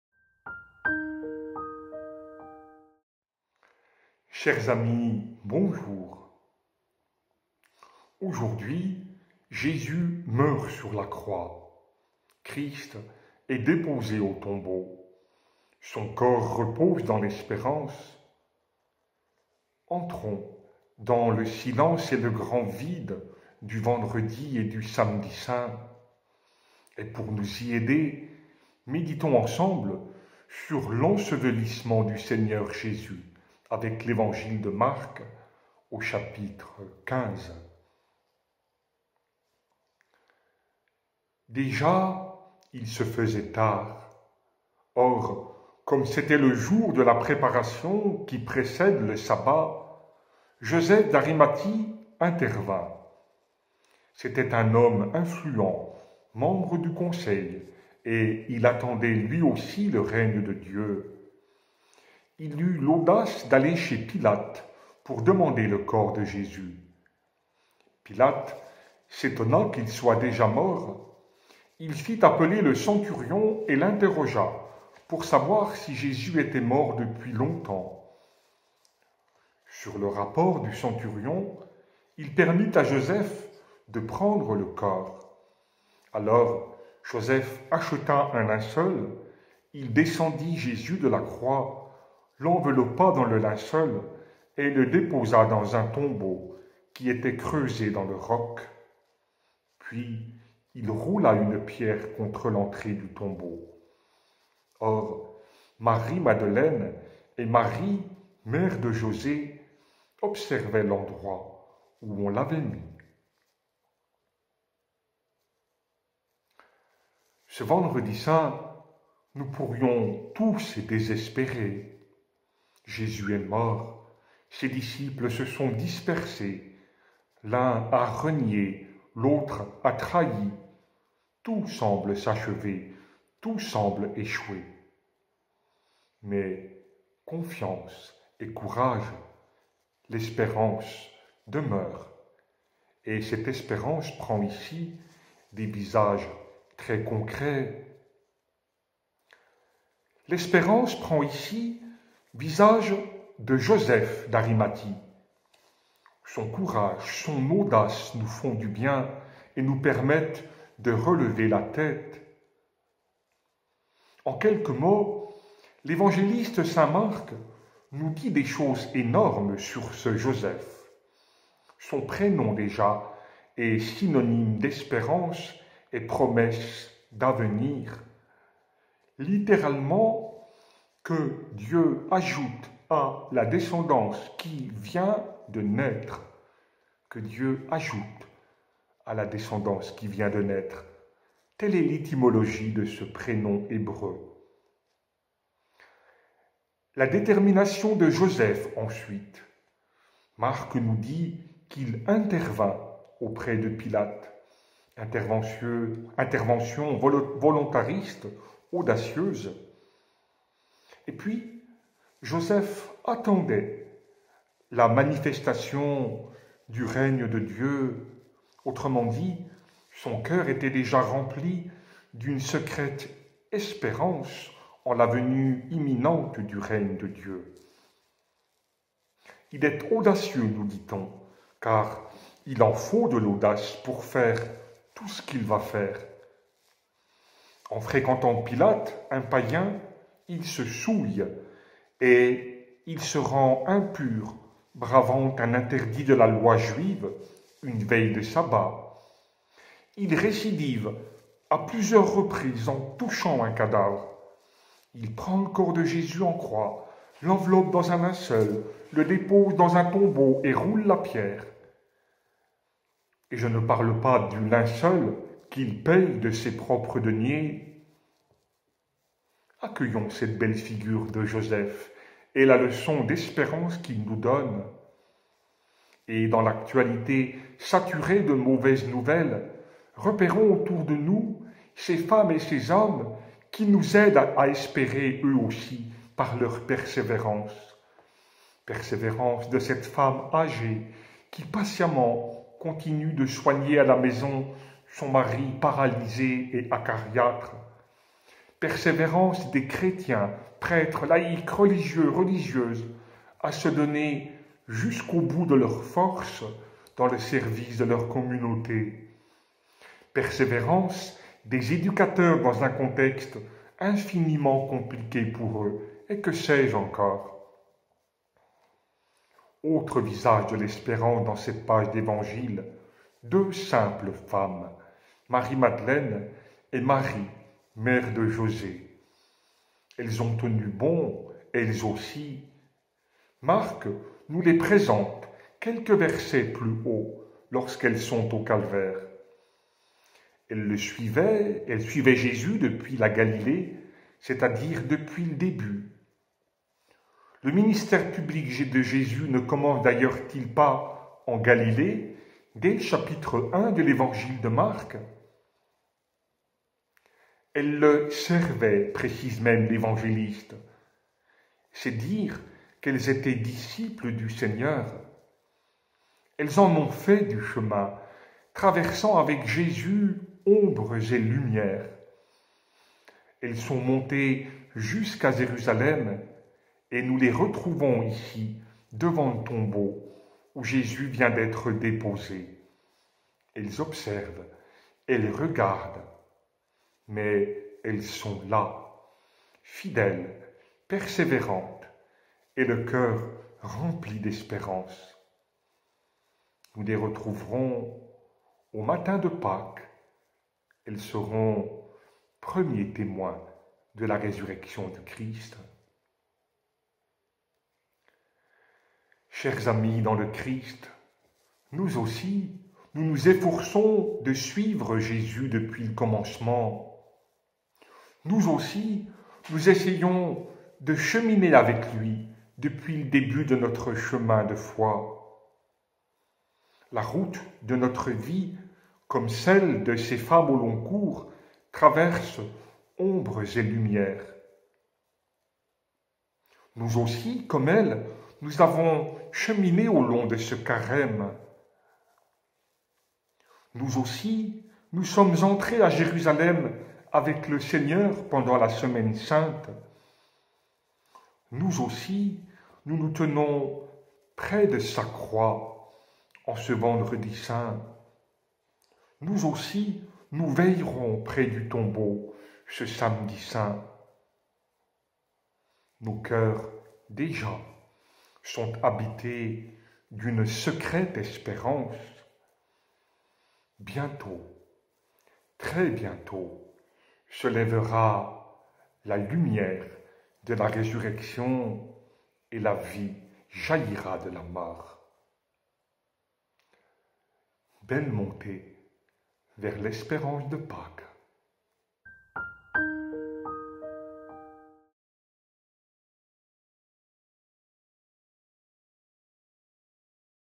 Récollection Semaine Sainte 2021 Vendredi​ Saint
En ce Vendredi Saint, le père Didier Sentenas nous aide à chercher des signes d'espérance, malgré tout. Ensevelissement Mc 15, 42 -47.